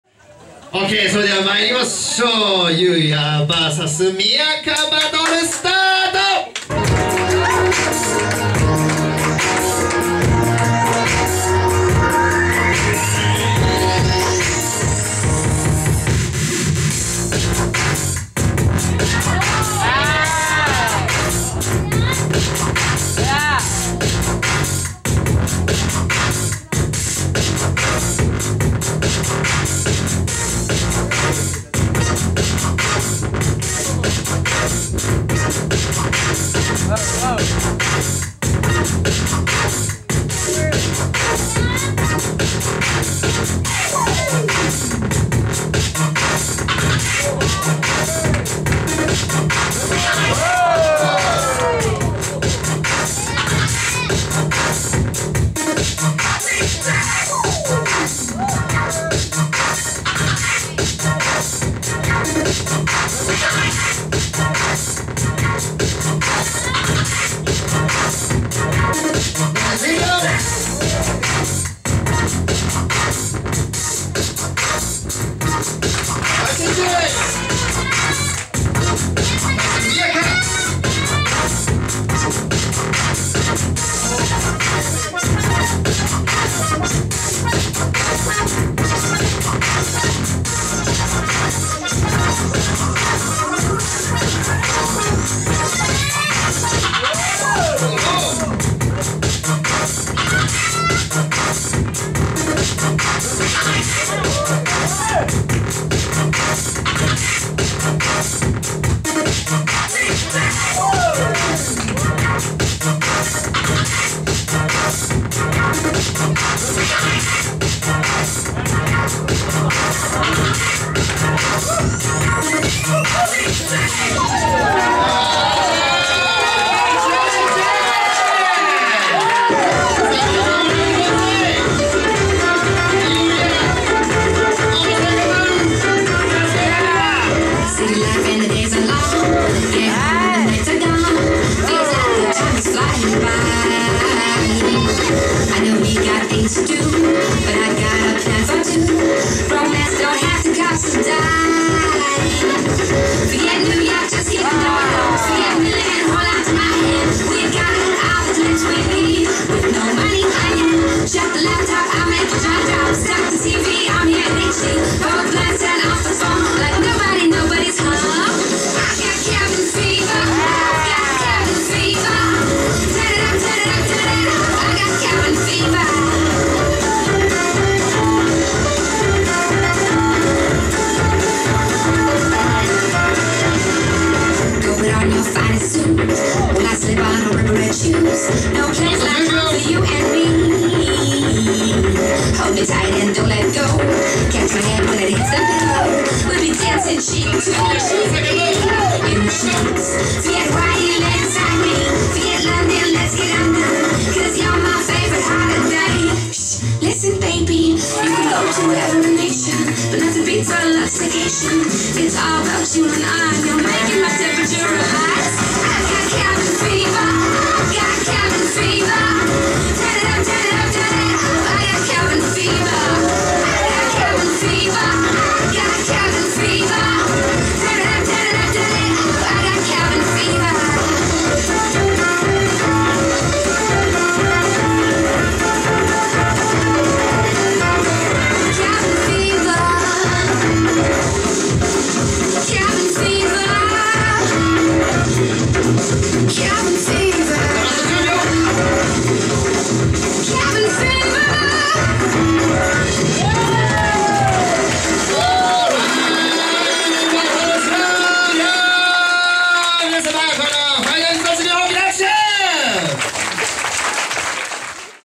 優弥(FORCE ELEMENTS) vs みやか DANCE@LIVE 2014 KIDS CHUBU vol.2【FINAL】
WIN :  みやか
Street dance Battle "DANCE@LIVE" in Japan.

sep. 29 2013 DANCE@LIVE 2014 KIDS CHUBU vol.2
@Pineapple Studio